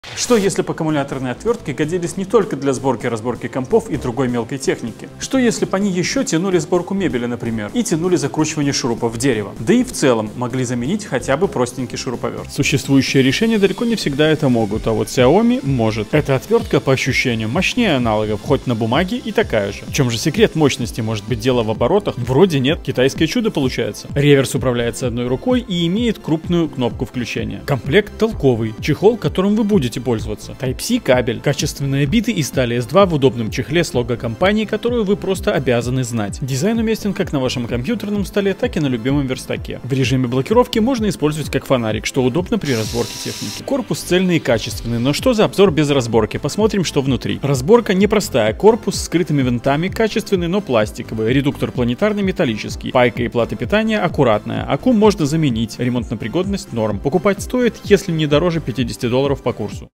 Китайское чудо или бесполезная вещь? Мне пригодится
Давайте немного отвлечемся от Казахстана...
Недавний короткий формат Shorts про аккумуляторные отвертки быстро набирает просмотры:
Поэтому, как и обещал, пару слов про отвертку от Xiaomi - мое мнение.Подытожу тут, так как минуты маловато:
Если вам нужен легкий шуруповерт или отвертка на каждый день для повседневные задач - сборка разборка бытовой техники, или сборка мебели - подойдет и будет радовать, вещь приятная.
Если же для профессиональных нужд - подойдет для сборки разборки техники или работы в СЦ и подобного. Для работы в ограниченных пространствах, конечно, лучше другая форма.
В целом - оставляет приятные ощущения от пользования. Осталось только собрать ее назад )
Лично я оставлю именно ее, а BD - подойдет, скорее, для работы в ограниченных пространствах.
Как подарок - идеальна.

В ролике использованы следующие инструменты:
1. Аккумуляторная отвертка BLACK+DECKER BD40K27 ION
2. Аккумуляторная отвертка BLACK+DECKER BCRTA01 USB HexDriver
3. Аккумуляторная отвертка Xiaomi Mi Electric Screwdriver Gun